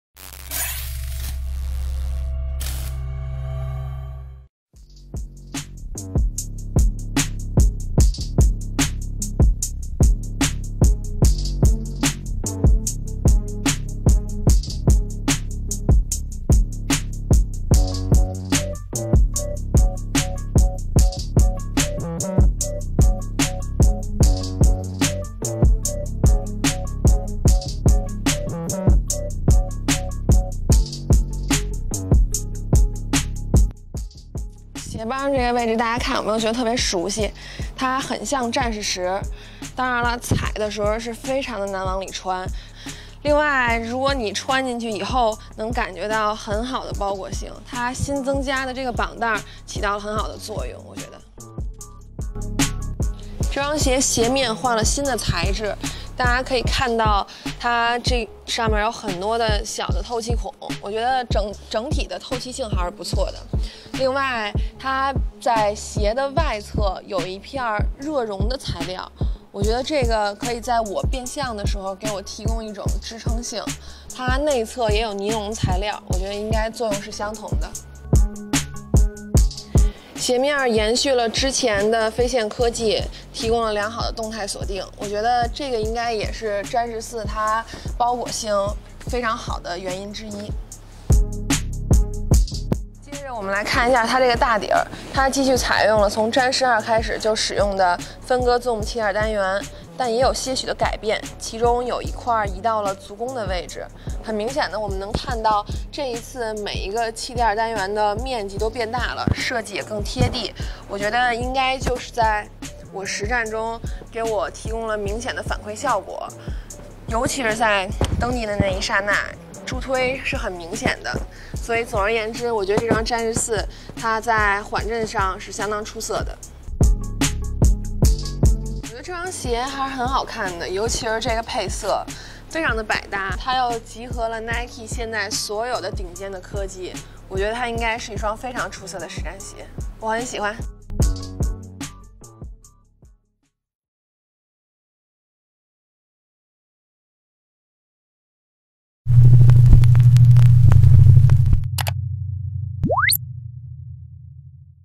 【极客鞋谈】詹姆斯14代实战测评 Nike Lebron James XIV
订阅【极客鞋谈】 帮我们粉丝到5000！
亚马逊购买Lebron James XIV